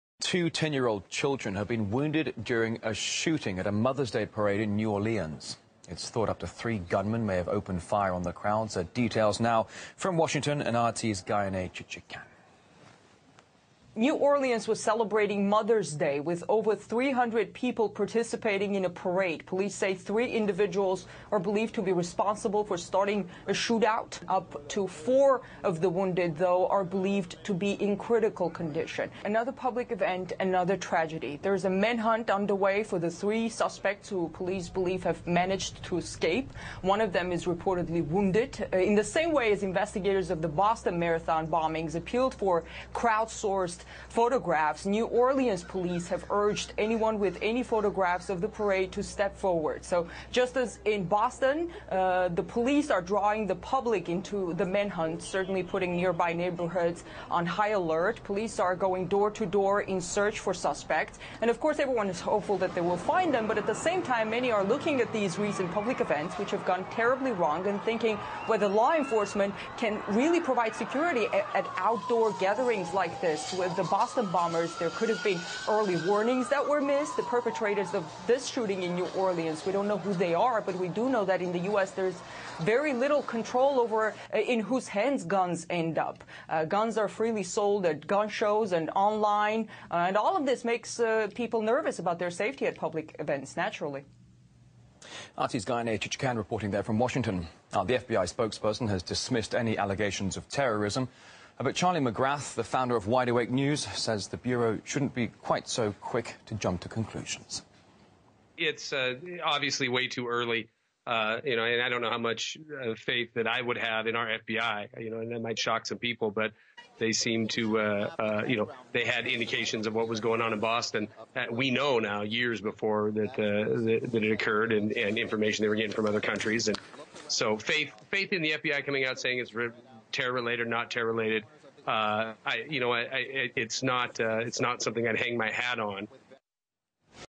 Parade Shooting = Gun Grab! @ 1:25
Forward  to 1:25 (to skip the bull) And you will hear the Real Reason for this FALSE FLAG!  Miss CHIC-HAK-YAN, Will tell you, "In Merka,Vee DO KNOO Dat Dare is bery leetal CONTROL over in hue's hans Guns End Up". "Guuns ar freely sould ene merca"  Can you tell me - Vat is dere rong wit dis Woman's who do dah nuse?